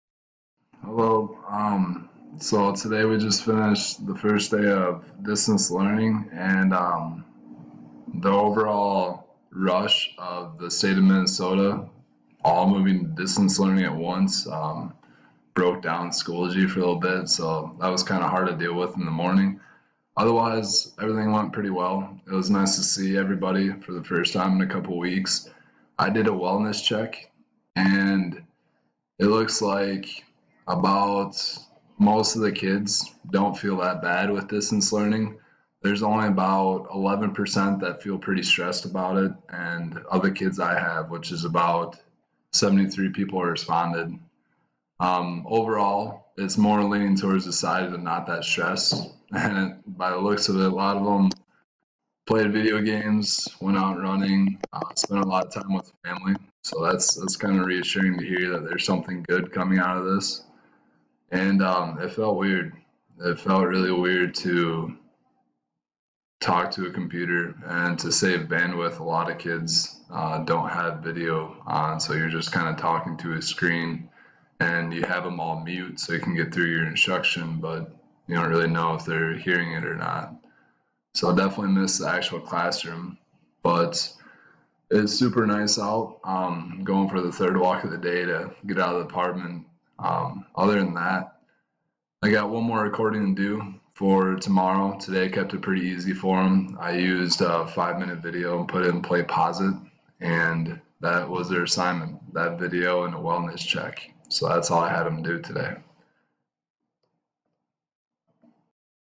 1st day of distance learning
March 30th marks the first day of distance learning due to COVID-19 and this video logs experience.